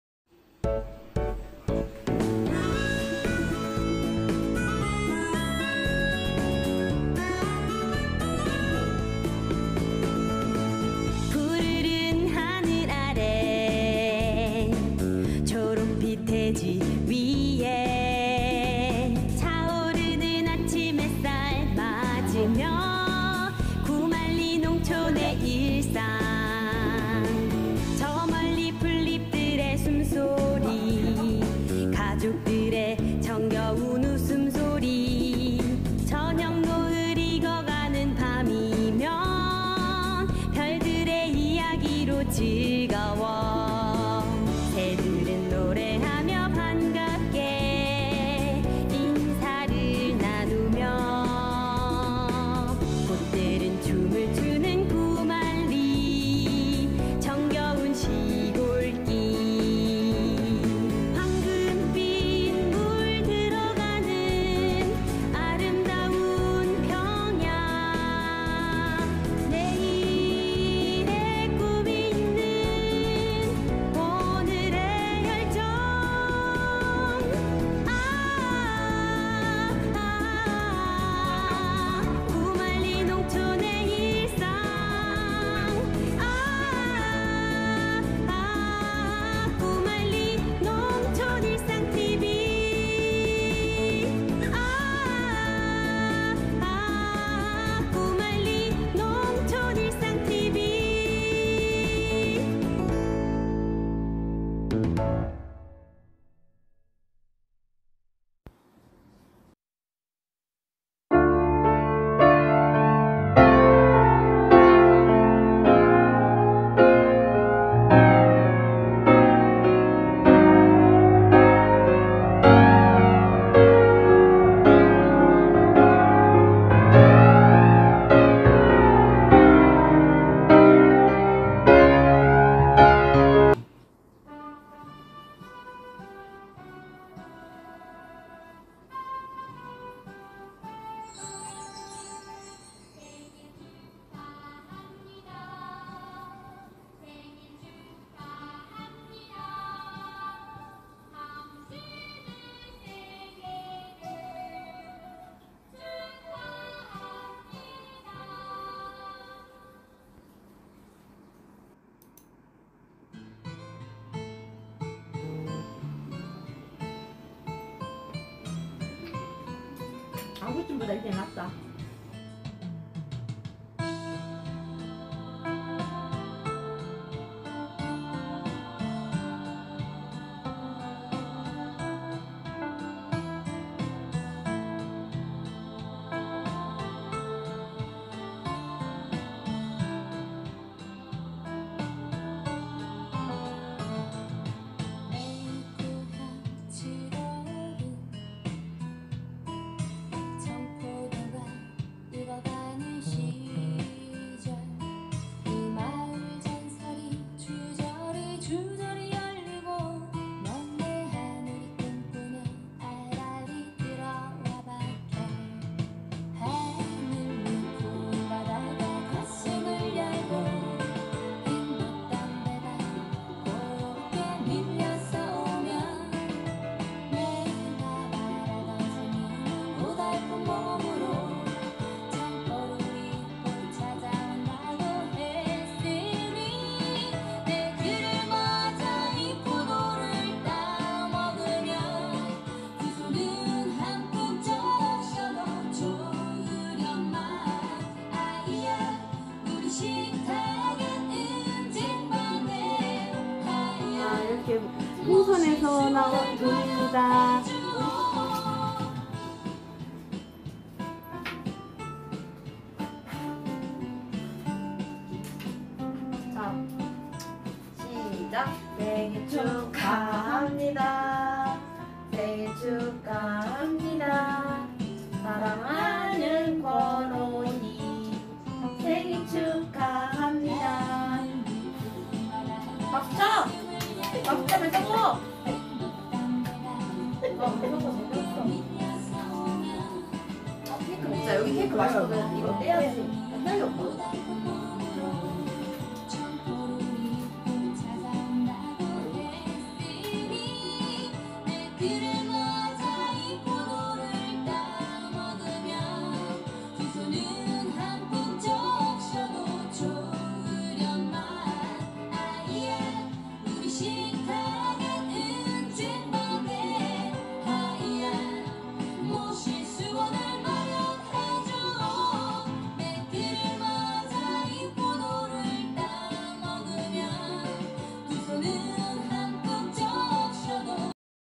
엄마생일
생일을 맞은 구만리농촌일상 권여사가 친구같고 연인같은
막내딸내미랑 생일이밴트를 합니다
취미가 전혀다른 아빠는 일끝나고
뒤늦식사를 하시고 우리 모녀는 편안히 이밴트를 합니다
또하편의 추억으로 영상을 기록합니다
생일축하합니다